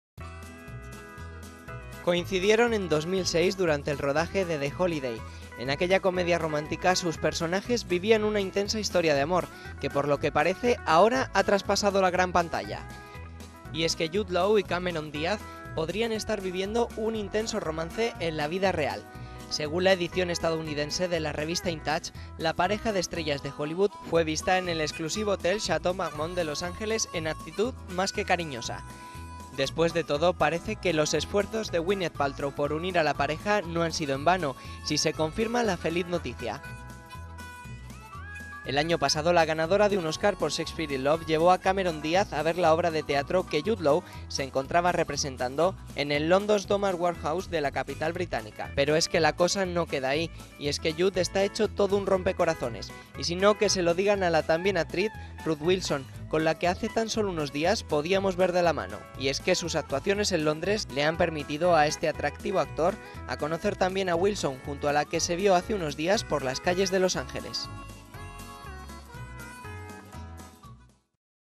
Jude Law y Cameron Diaz podrían estar saliendo
Coincidieron en 2006 durante el rodaje de 'The Holiday'. En aquella comedia romántica, sus personajes vivían una intensa historia de amor que, por lo que parece y según afirma la revista 'In Touch', ahora ha traspasado la gran pantalla.